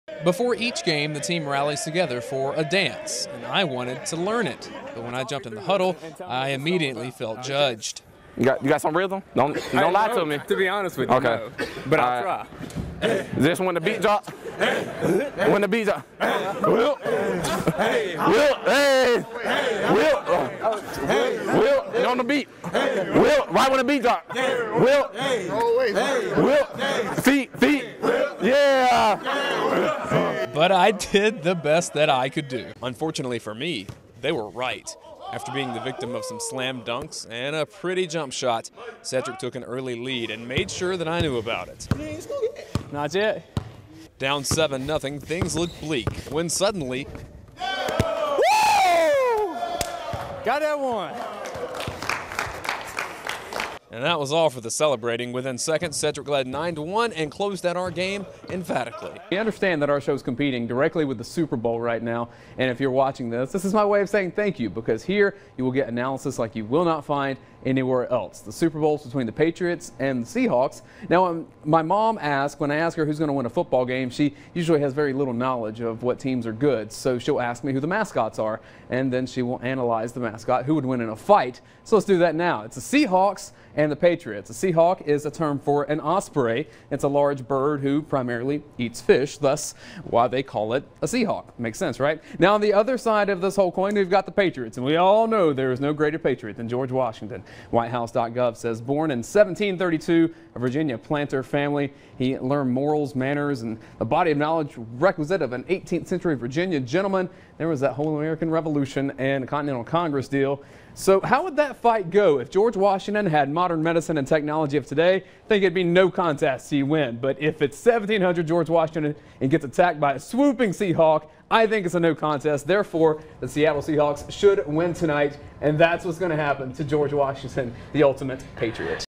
WRBL's Dayne Young on-air personality
Some example's of Dayne Young's personality on air with WRBL